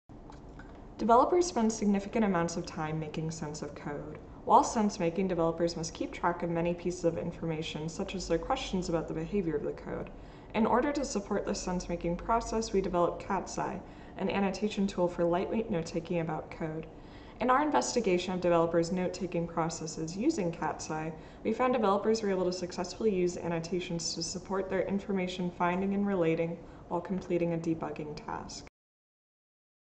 Using Annotations for Sensemaking About Code
Amber Horvath, Brad A Myers, Andrew Macvean, Imtiaz Rahman

UIST 2022: The ACM Symposium on User Interface Software and Technology
Session: Programming, Kits, and Libraries

Developers spend significant amounts of time finding, relating, navigating, and, more broadly, making sense of code.
While sensemaking, developers must keep track of many pieces of information including the objectives of their task, the code locations of interest, their questions and hypotheses about the behavior of the code, and more.
Despite this process being such an integral aspect of software development, there is little tooling support for externalizing and keeping track of developers' information, which led us to develop Catseye -- an annotation tool for lightweight notetaking about code.
Catseye has advantages over traditional methods of externalizing code-related information, such as commenting, in that the annotations retain the original context of the code while not actually modifying the underlying source code, and can support richer interactions such as lightweight versioning, following-up on the annotation content, and can be used as navigational aids.
In our investigation of developers' notetaking processes using Catseye, we found developers were able to successfully use annotations to support their code sensemaking when completing a debugging task, with participants in a small study who used Catseye fixing more bugs, on average, than the baseline.

Video previews for papers at UIST 2022